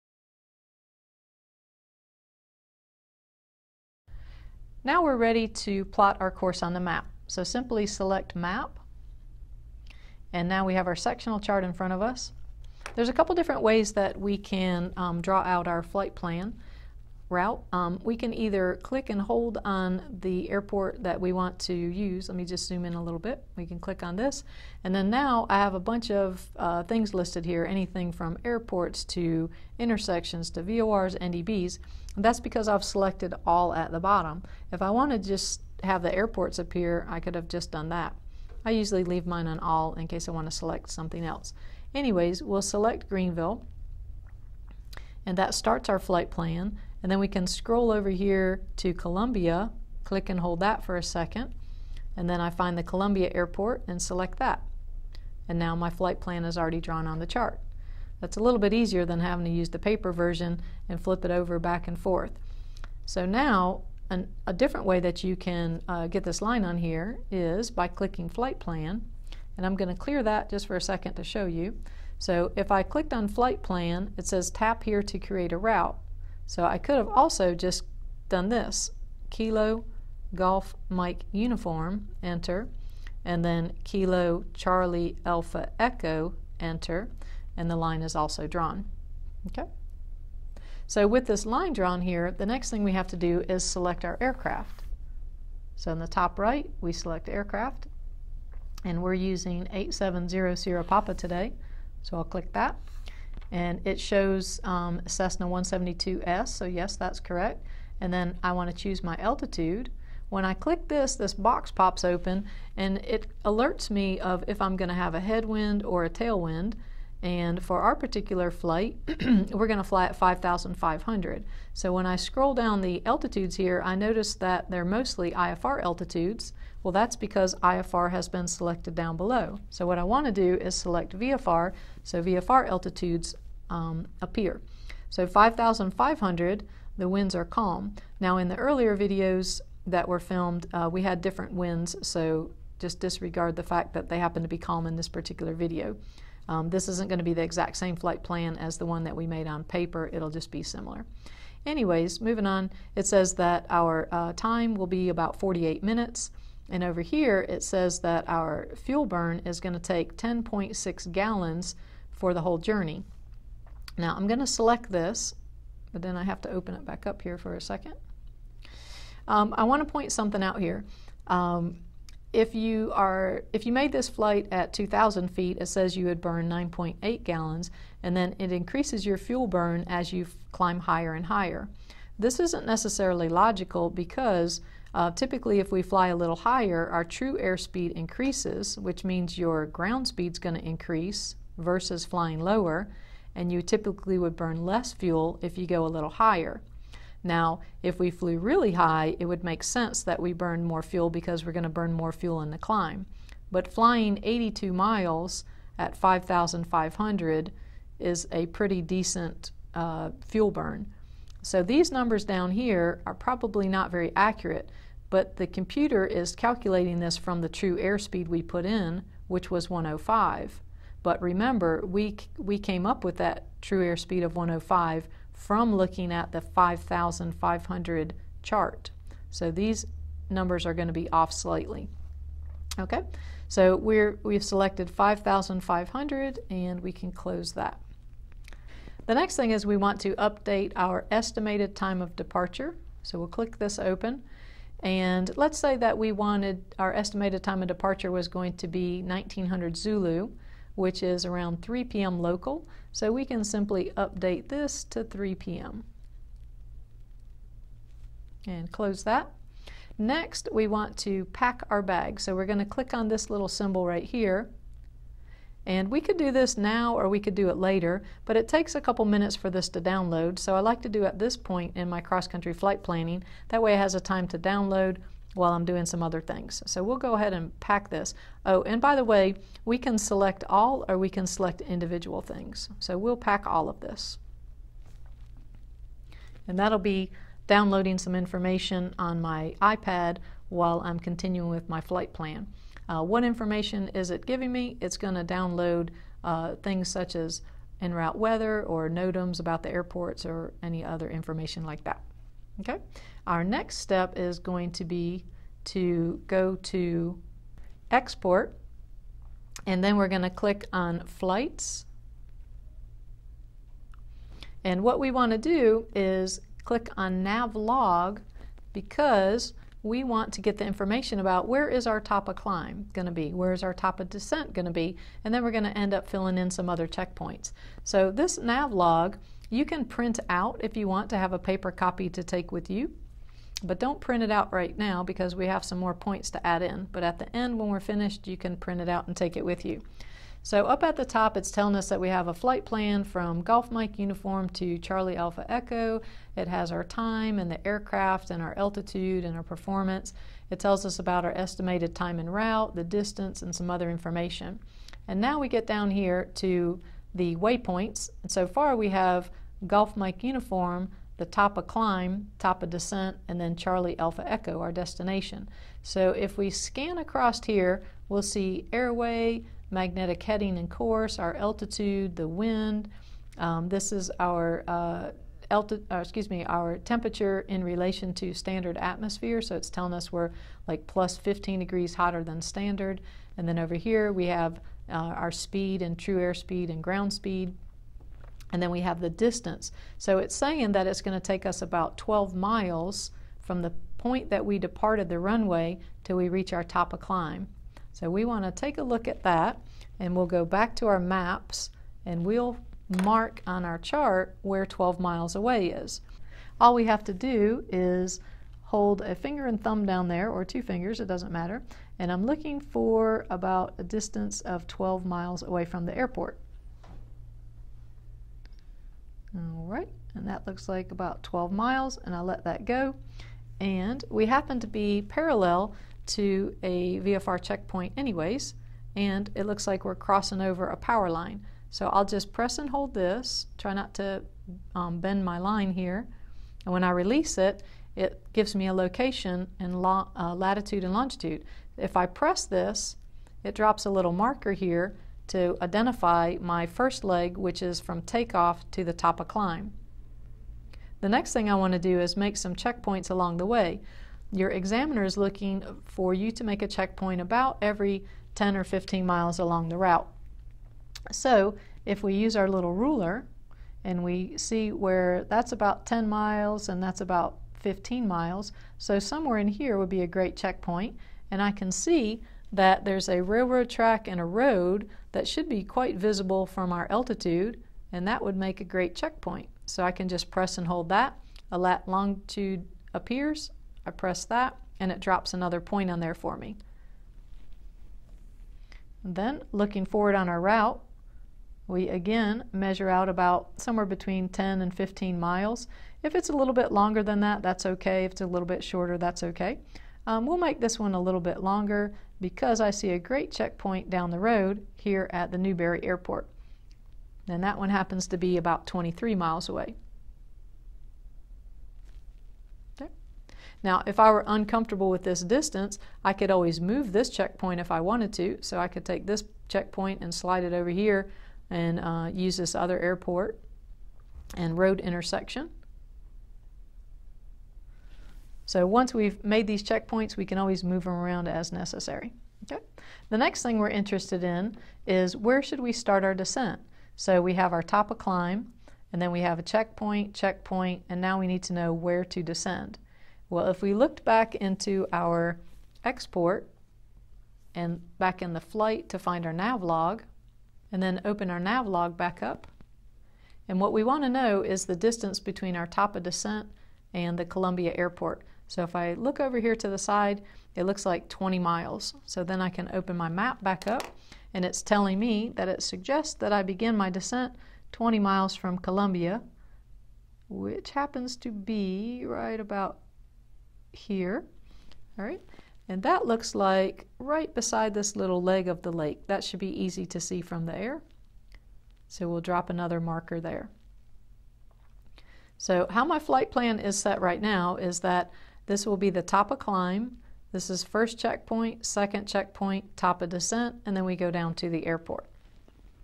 Flight Planning with ForeFlight Part 2 - XC Flight Planning (Private Pilot Lesson 14cc)
Second of 3 videos explaining how to flight plan using an electronic flight bag (EFB), using ForeFlight for demonstration purposes.

Thinking of becoming a pilot?